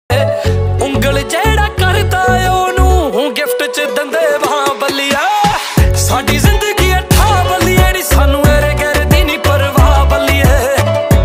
Punni Combine Harvester Machine || Multi Crop Combine Harvester With Straw Making Facility! ❤️
Get ready to witness the incredible sight of Punni's combine harvester dominating the road in this mesmerizing video! As Punni fearlessly maneuvers the powerful machine, you'll be captivated by the symphony of efficiency and precision. Watch in awe as the golden harvest sways in the wind, perfectly captured in this thrilling YT Shorts creation.